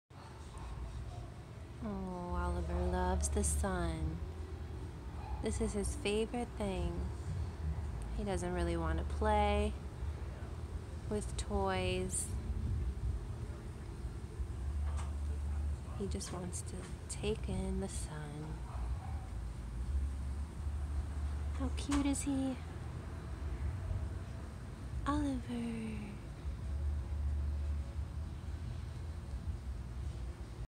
ADOPTABLE: This Sunbathing Beauty is Oliver
Oliver is available for adoption. He's also open to being fostered.